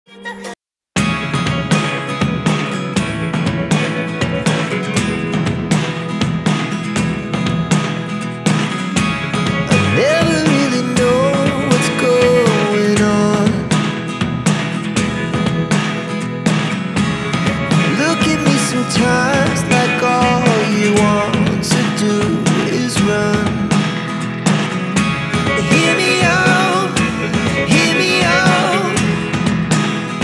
Aerial Tour of CGC Jhanjeri: Where Modernity Meets Nature's Splendor!
Discover the breathtaking beauty of CGC Jhanjeri from new heights! 🚁✨ Soar above our campus with stunning drone shots that showcase the perfect blend of modern infrastructure and lush green surroundings. Experience education from a whole new perspective.

Admissions are closing soon. Grab your spot now.